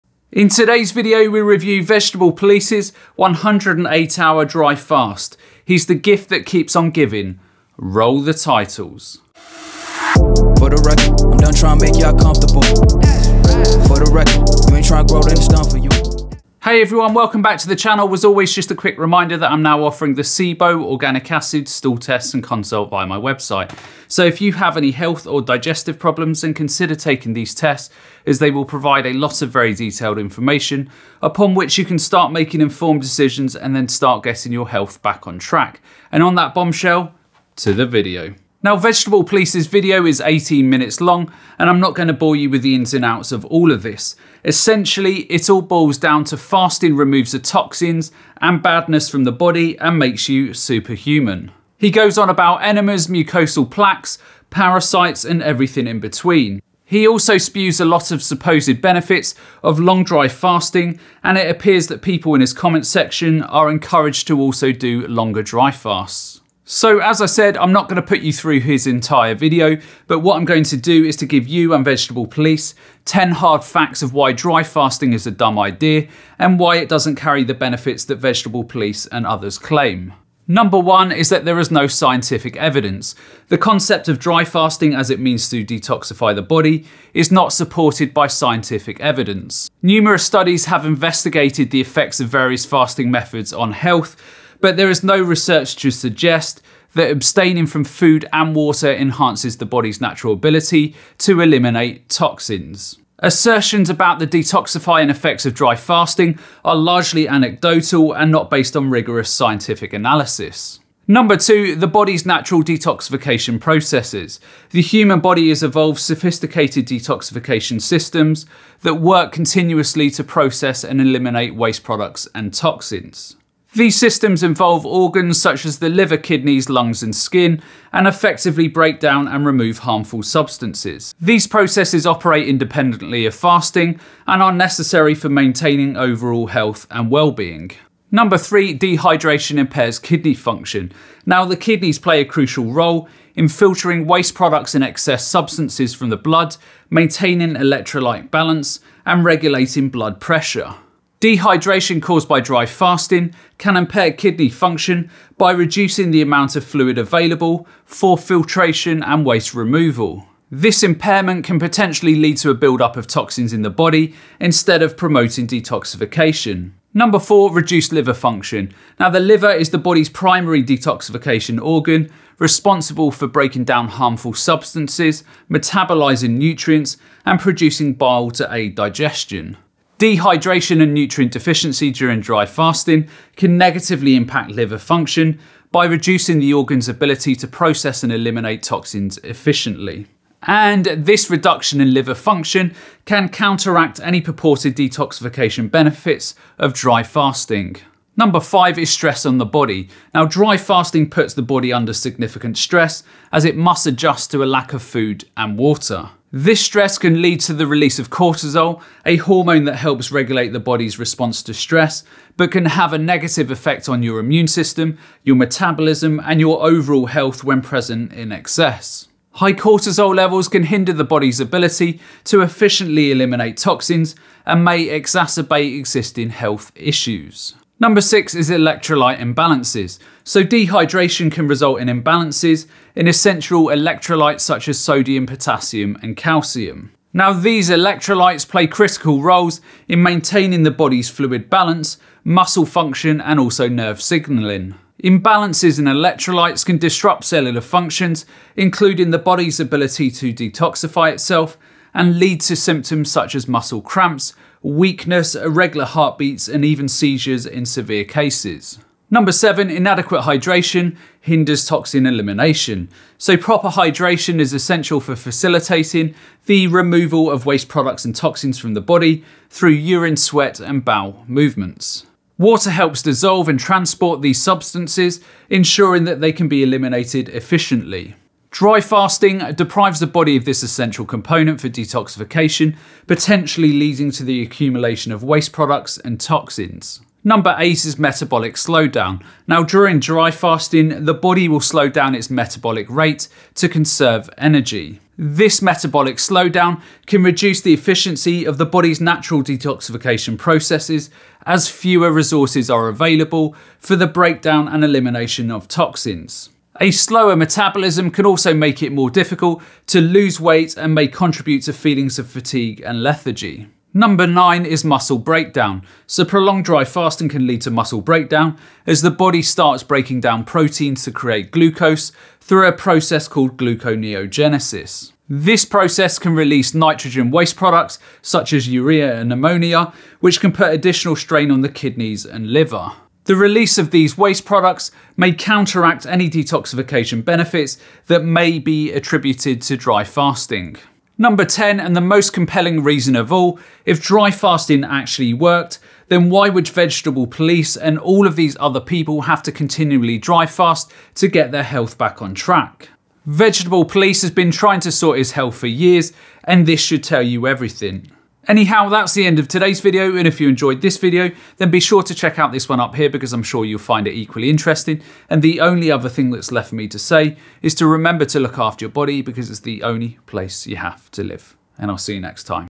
Debunking Vegetable Police's 108 Hour Dry Fast!
In today's video I debunk Vegetable Police's 108 hour dry fast. What really happened to his body during this time – let's find out.

As always if you have a question for me then askgojiman in the comments below or send your video question through to